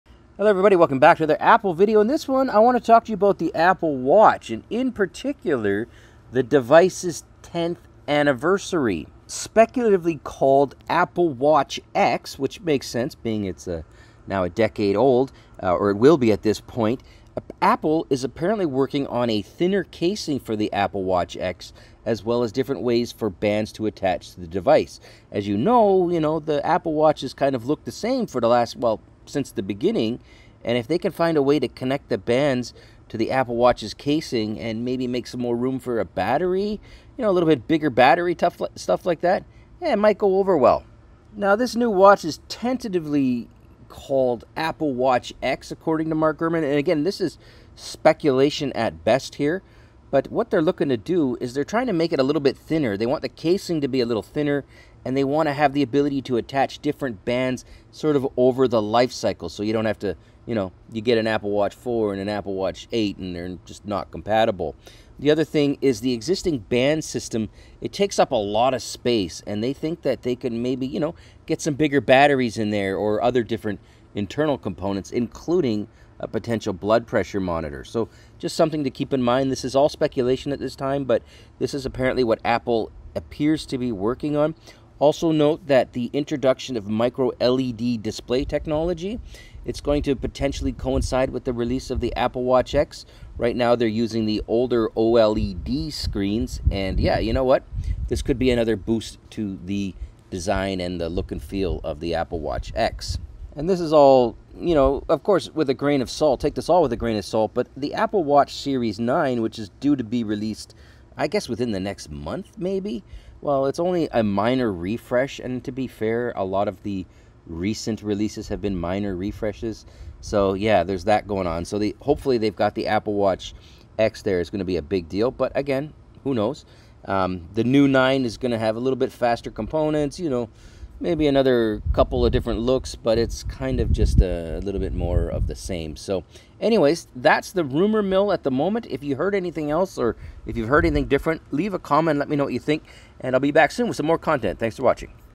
Apple Watch X - 10th Anniversary Speculations - Magnets, BPM Monitor, and More!
Hey there, Apple enthusiasts! Welcome back to another video. Today, we're diving deep into the speculations surrounding the Apple Watch, especially as we approach its 10th Anniversary. Could we see an all-new Apple Watch X? Let's discuss!

🕒 Timestamps:
0:00 - Introduction & Importance of 10th Anniversary
0:09 - Speculations about the Apple Watch X
0:25 - The iconic look of the Apple Watch over the years
0:41 - The potential name "Apple Watch X" and its thinner design
1:05 - Possible changes in the band system
1:24 - Introduction of Micro-led display technology in Apple Watch X
1:43 - Reminder: Always take rumors with a grain of salt
1:47 - The upcoming Apple Watch series 9 - what to expect
2:05 - Wrapping up and engaging the community
2:24 - Outro

💡 Video Overview:

We discuss the possible name, "Apple Watch X," marking its 10th anniversary.
Will Apple be introducing a thinner casing for the Apple Watch X? And what's the deal with new ways to attach bands?
There's a chance the watch could feature a Micro-led display, switching from the older OLED screens.
Speculations about the new watch including a potential blood pressure monitor.
Quick touch on the soon-to-be-released Apple Watch series 9 and what we might expect.
As always, remember that these are rumors, and while they're exciting, it's essential to wait for official announcements.
💬 Join the Discussion: Have you heard any other rumors or have a different opinion? Share in the comments below, and let's get a conversation started!

Thanks for tuning in, and I'll see you soon with more updates and reviews. Until then, stay tech-savvy! ✌️